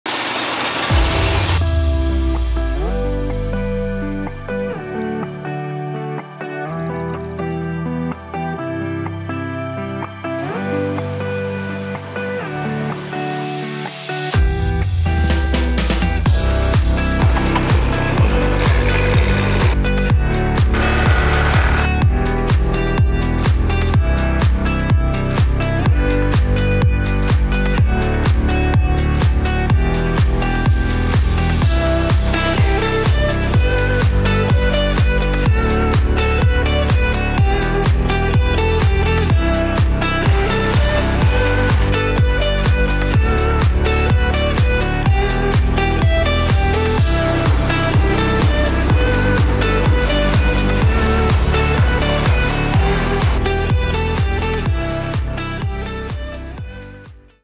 Demodag Biodiversiteit en Techniek 2024
Cumela organiseerde samen met BoerenNatuur Veluwe en BoerenNatuur Utrecht Oost voor de eerste keer een Demodag Biodiversiteit en Techniek. Op deze dag konden opdrachtgevers, cumelabedrijven, boeren en andere geïnteresseerden kennismaken met de mogelijkheden om met techniek de biodiversiteit te helpen. Hier vind je een terugblik van deze dag.